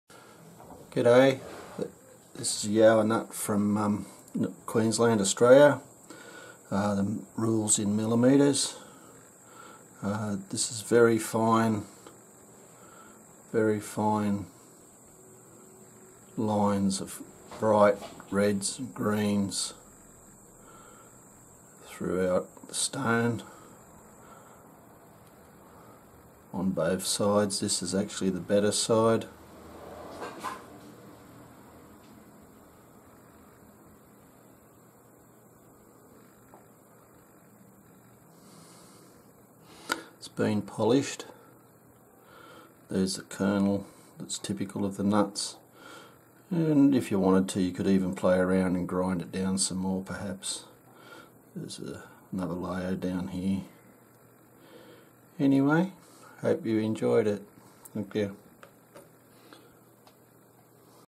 Australian Boulder Opal Yowah Opal Nut polished Speci 668
Australian Boulder Opal Yowah Opal nut polished Specimen . A great example showing the internal Kernal which is so typical of these nuts